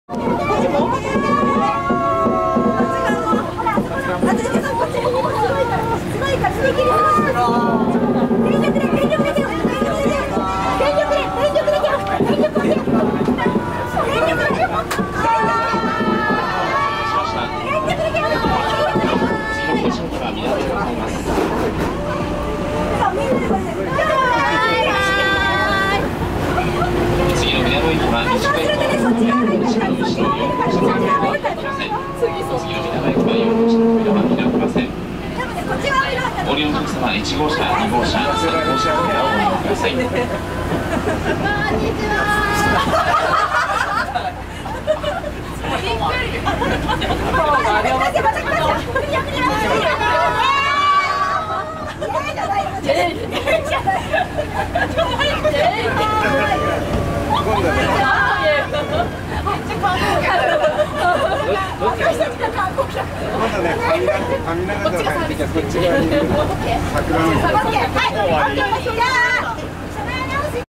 秩父鉄道SL×ハンコミ！
2017年6月3日実施。
車内より全力で手を振る。